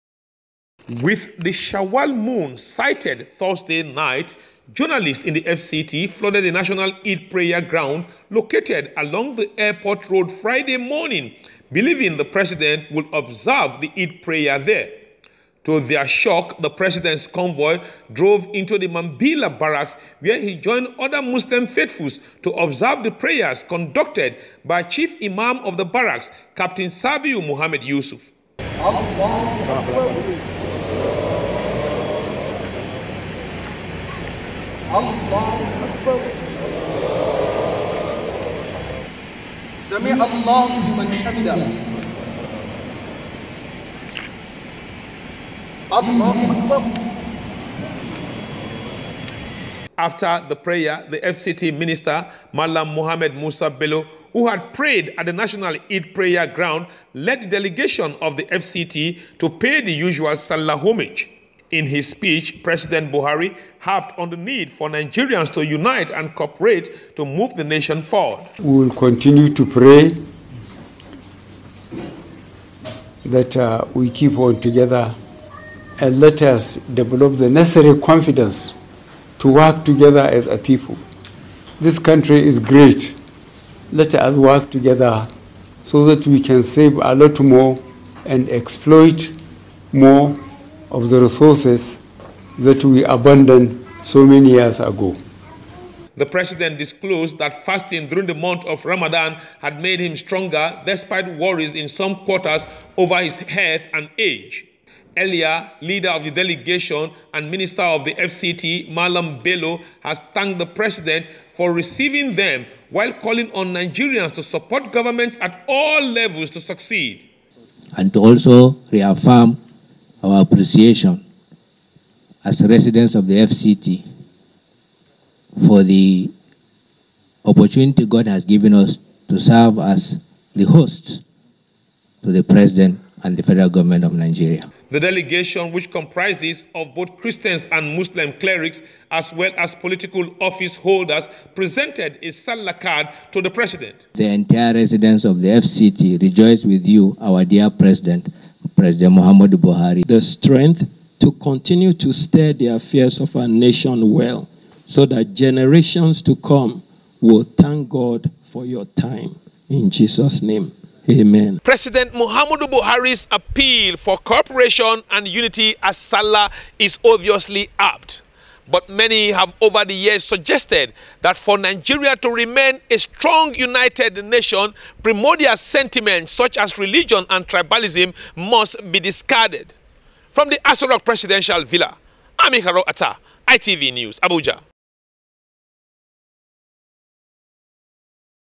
Pres. Buhari Calls For Greater Unity Cooperation
PRESIDENT MUHAMMAD BUHARI HAS AGAIN APPEALED TO NIGERIANS  TO UNITE AND WORK CLOSELY IF THE NATION MUST REACH AN ENVIABLE PLACE IN THE LEAGUE IF NATIONS

THE PRESIDENT SPOKE WHEN HE HOSTED DELEGATION FROM THE FCT THAT PAID HIM THE TRADITIONAL SALLAH HOMAGE.